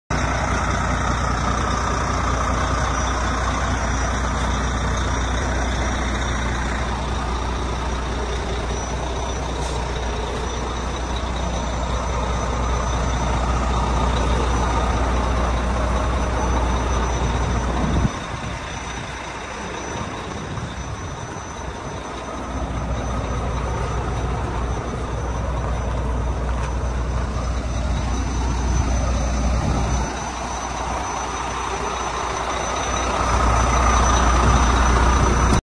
2016 VOLVO VNL64T670 For Sale
AUTOMATIC!!!  SLIDING 5TH WHEEL, TRAILER BRAKE, LIQUID DEF,